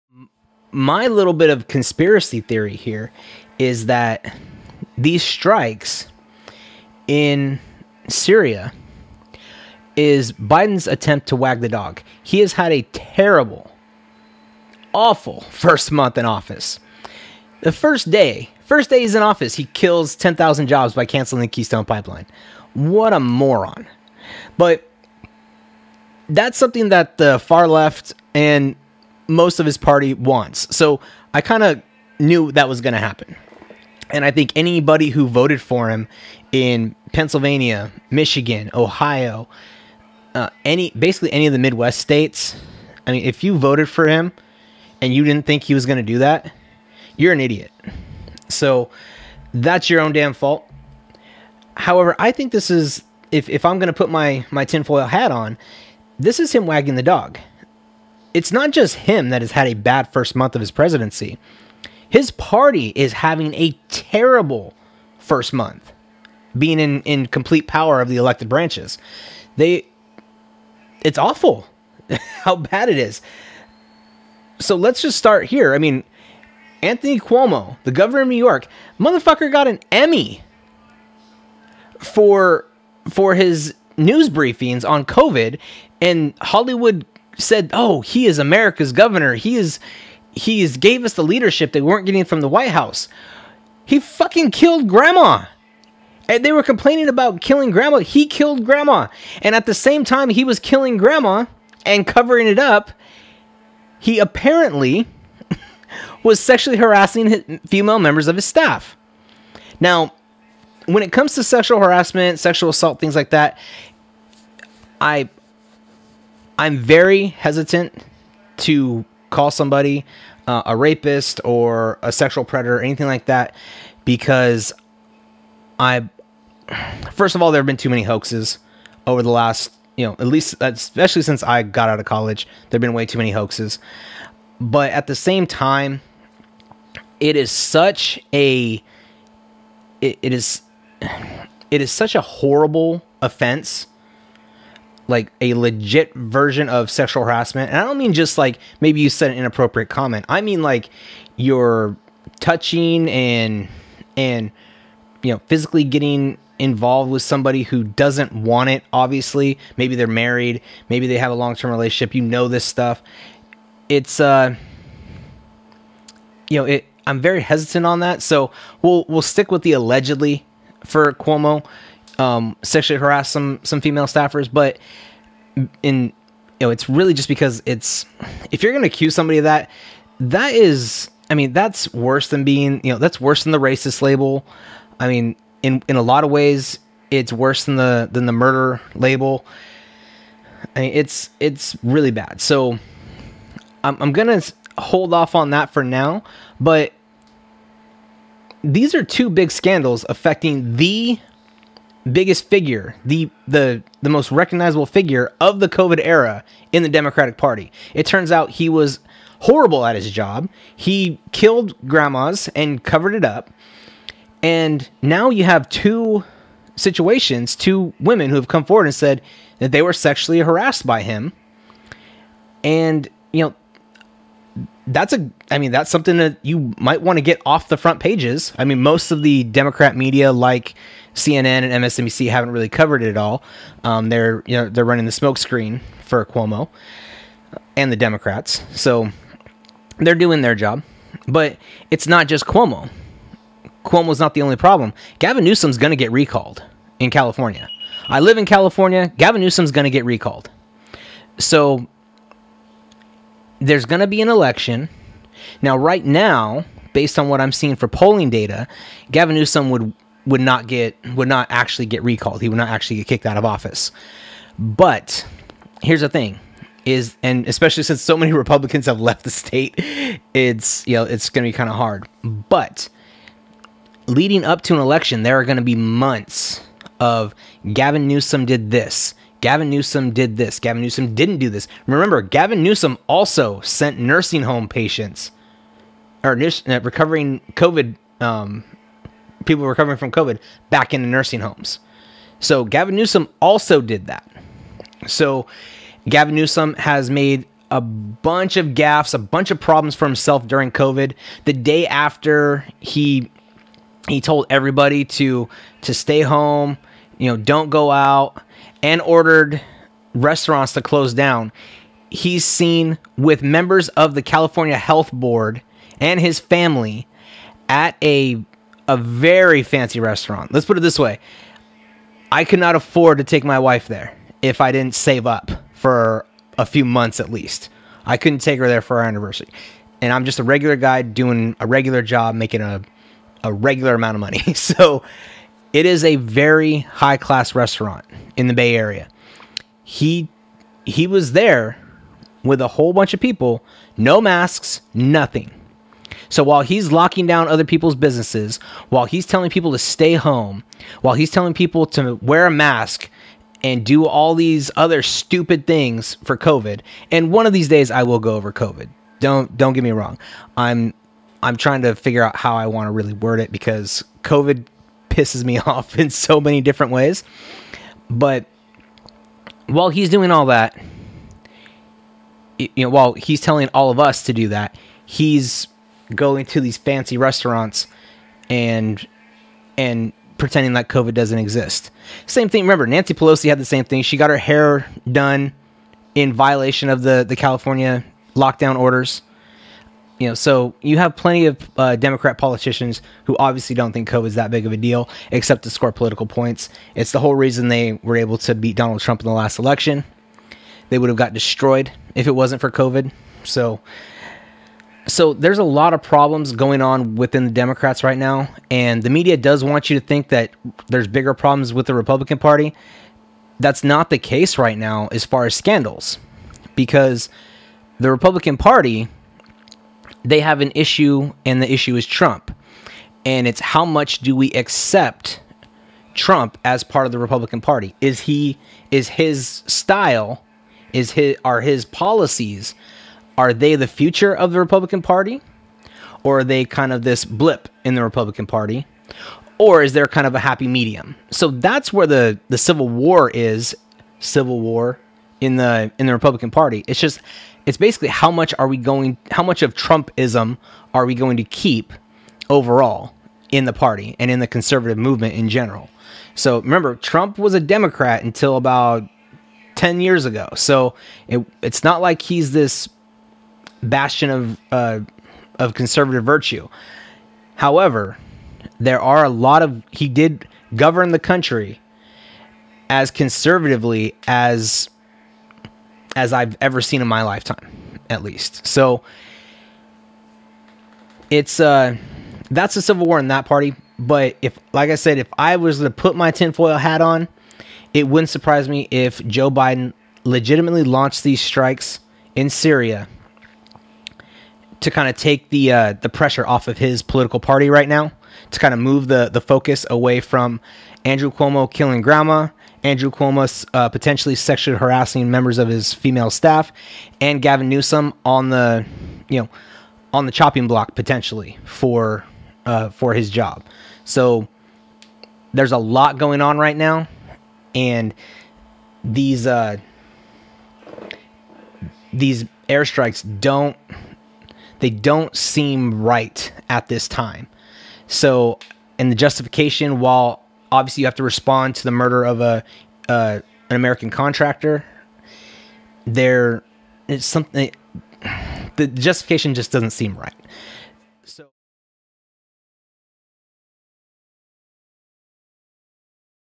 CONSPIRACY THEORY TIME | Did Biden Bomb Syria To Cover For Democrat Scandals a.k.a. "Wag The Dog"?
Kirk discusses the conspiracy theory he has about Biden bombing Syria.